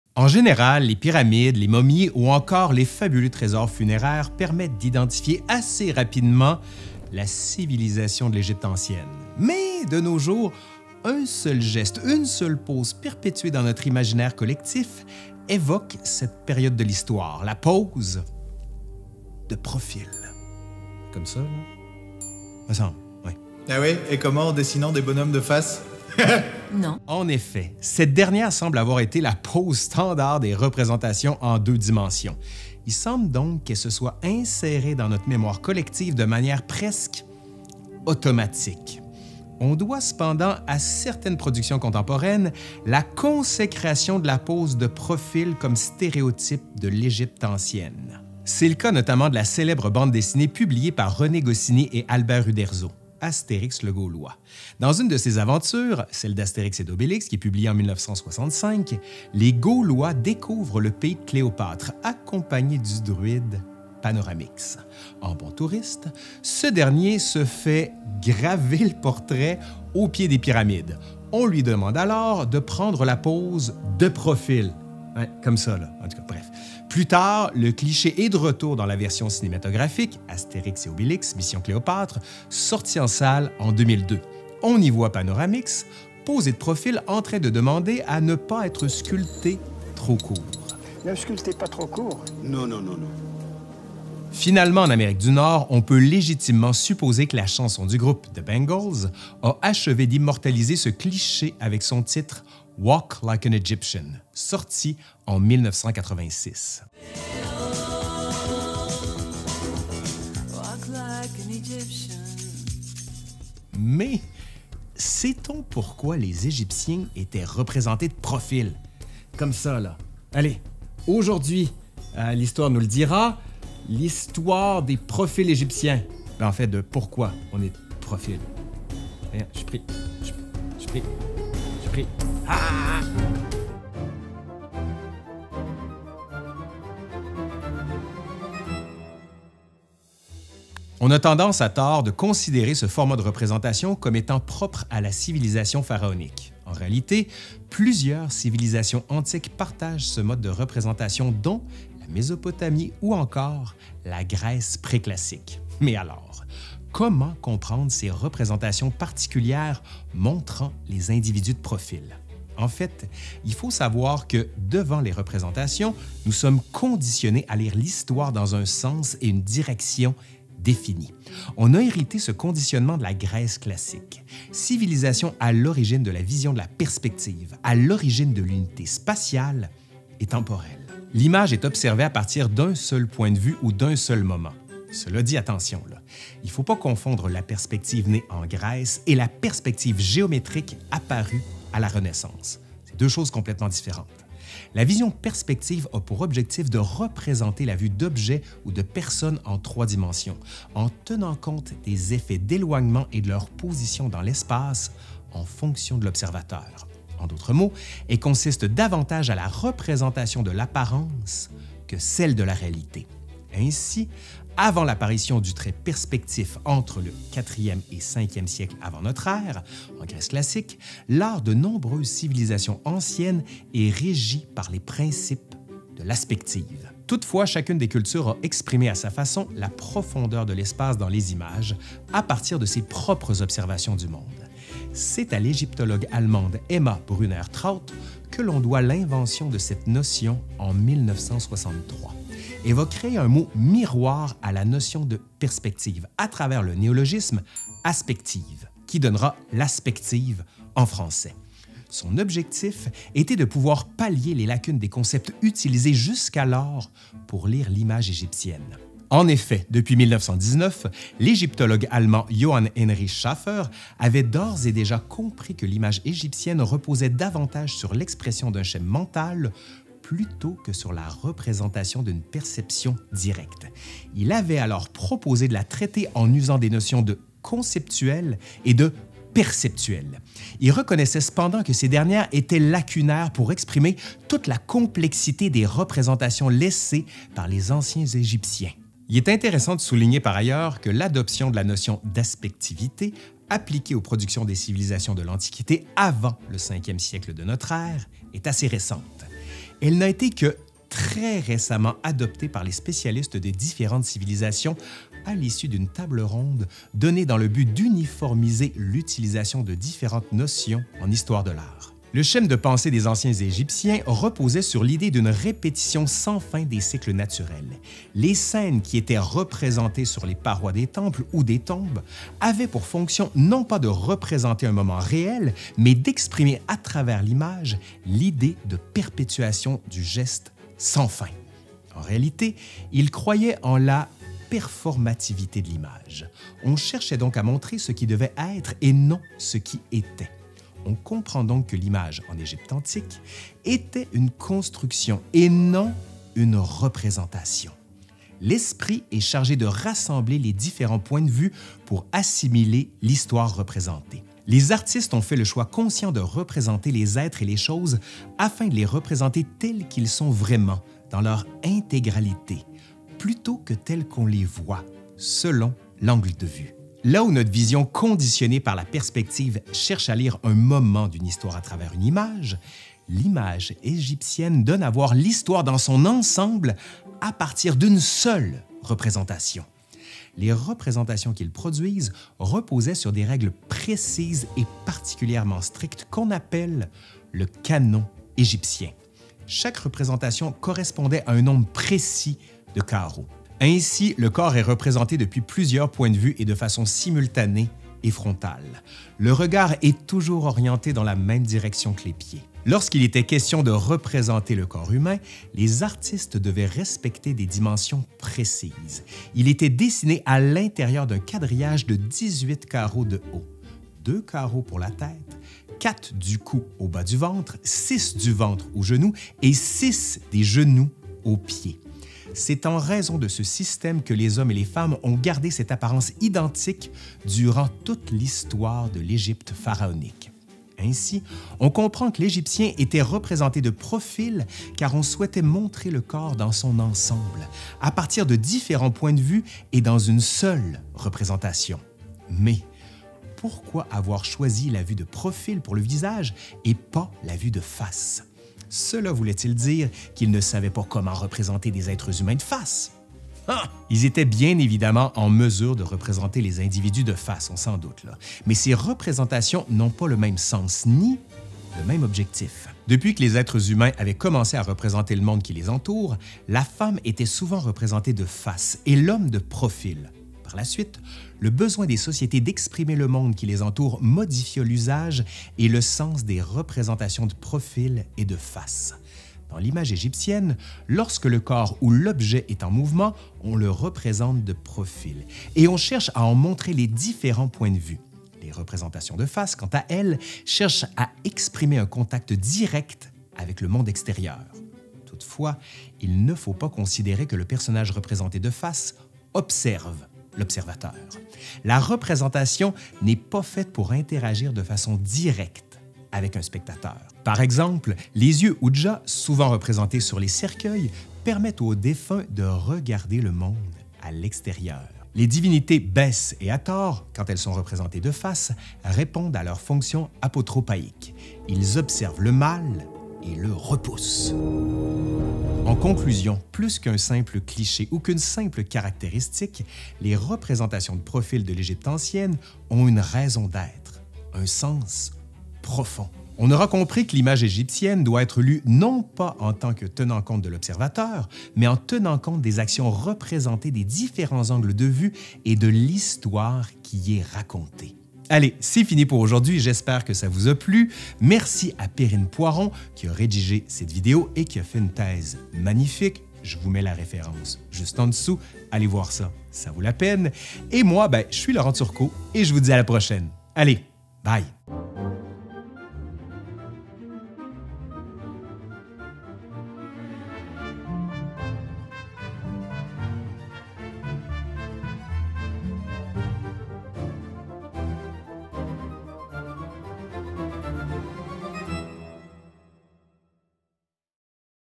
Pourquoi les Égyptiens sont toujours de profil ? | L'Histoire nous le dira # 210
En général, les pyramides, les momies ou encore les fabuleux trésors funéraires permettent d’identifier assez rapidement la civilisation de l’Égypte ancienne. Pourtant, de nos jours, un seul geste, une seule pose perpétuée dans notre imaginaire collectif évoque cette période de l’histoire : la pose de profil.

Pour soutenir financièrement la chaîne, au choix:
1. Cliquez sur le bouton « Adhérer » sous la vidéo.

Avec: Laurent Turcot, professeur en histoire à l'Université du Québec à Trois-Rivières, Canada

Sources et pour aller plus loin:
G. Robins, Proportion and Style in Ancient Egyptian Art, Austin, 1994
E. Warmenbol (ed.), Beautés d'Égypte. "Celles que les ans ne peuvent moissonner", catalogue d'exposition, Treignes, 2002, p. 25-32
C. Barbotin, « Aspective et perspective dans la pensée égyptienne de l’histoire », Pallas, 105, 2017, 27-39.
D. Laboury, « De l’individualité de l’artiste dans l’art égyptien », dans G. ANDREU (éd.), L’art du contour. Le dessin dans l’Égypte ancienne, Paris, 2013, p. 36-41
D. Laboury, « L’artiste égyptien, ce grand méconnu de l’Égyptologie », dans G. ANDREU (éd.), L’art du contour. Le dessin dans l’Égypte ancienne, Paris, 2013, p. 28-35
D. Laboury, "Portrait versus Ideal Image", dans Willeke WENDRICH (ed.), UCLA Encyclopedia of Egyptology (peer reviewed online publication), Los Angeles (2010)
R. Tefnin, « Regard de face-regard de profil. Remarques préliminaires sur les avatars d’un coupe sémiotique », Annales d’Histoire de l’art et d’archéologie 17, 1995, p. 7-25.
V. Angenot, “Cadre et organisation de l'espace figuratif dans l'Égypte ancienne”, in Thierry Lenain and Rudy Steinmetz (eds), Cadre, seuil, limite. La question de la frontière dans la théorie de l’art, Brussels: La Lettre Volée, 2010: 21-50
E. Oulié, « Introduction générale », Pallas, L’aspective dans l’art antique / Oppida et ‘civilisation des oppida’, vol. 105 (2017), 15-25.
C. Mathieu (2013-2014) "Rencontre entre art et écriture. À la découverte des hiéroglyphes et du dessin égyptien", Dossier pédagogique

Le texte et les sources ont été vérifiés de manière indépendante par un historien PhD.